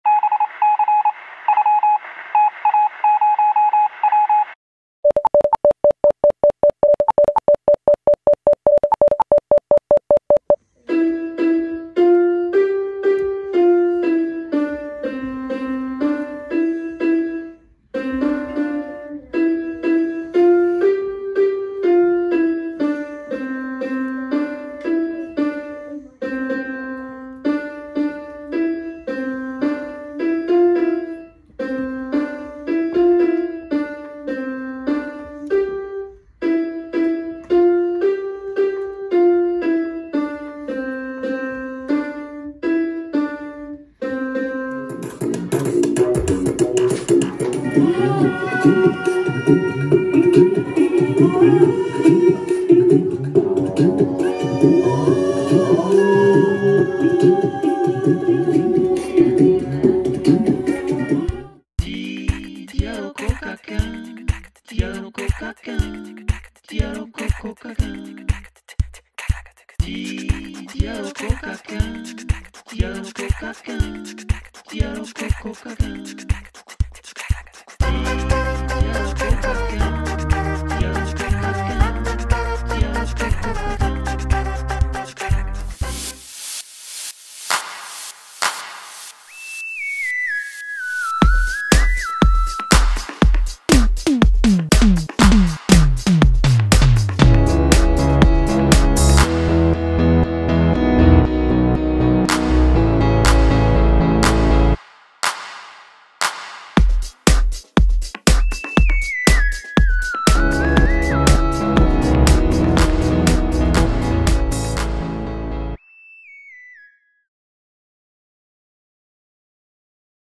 Diwrnod Miwsig Cymru !
Mae pawb yn medru cael hwyl gyda Technoleg Cerdd does dim rhaid cael arbenigedd.